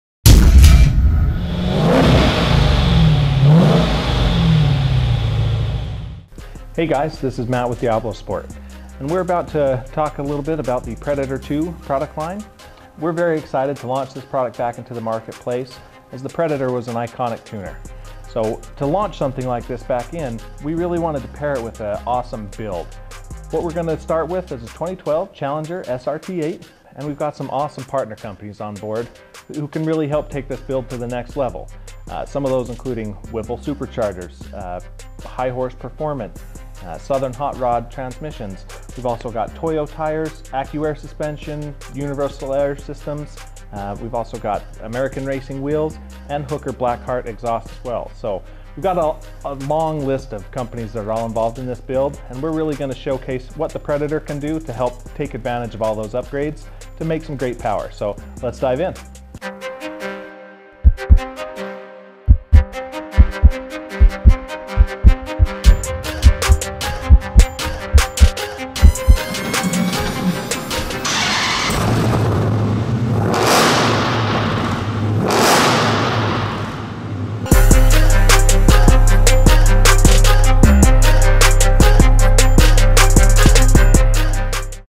DiabloSport: Project Predator SRT
We're kicking off the Project Predator SRT build!  We will showcase how the Predator tuning combined with some of the best aftermarket products available can turn this Hemi powered Dodge into a Predator!